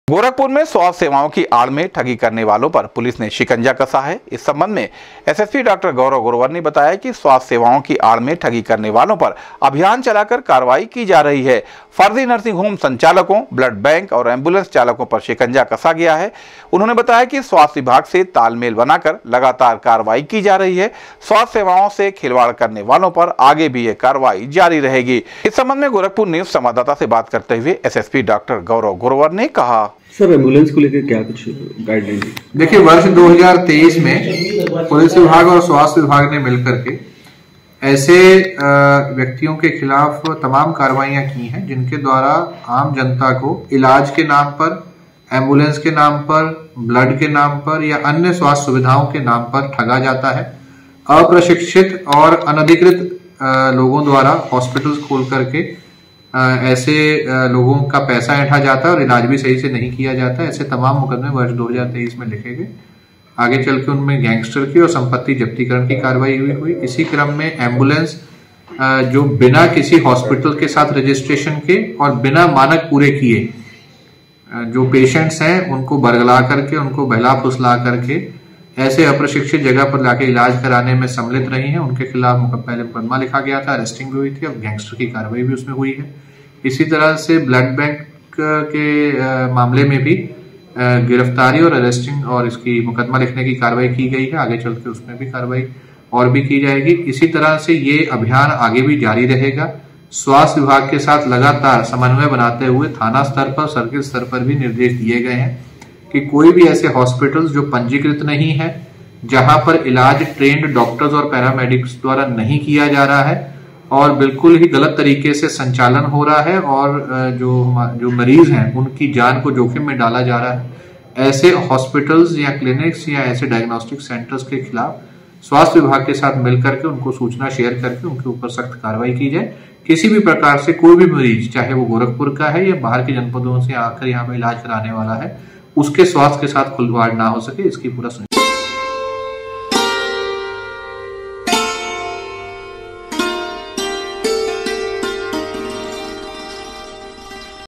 गोरखपुर में स्वास्थ्य सेवाओं की आड़ में ठगी करने वालों पर पुलिस ने कसा शिकंजा
गोरखपुर में स्वास्थ्य सेवाओं की आड़ में ठगी करने वालों पर पुलिस ने कसा शिकंजा, एसएसपी डॉक्टर गौरव ग्रोवर ने कहा, स्वास्थ्य सेवाओं की आड़ में ठगी करने वालों पर अभियान चलाकर की जा रही हैं कार्रवाई,,,,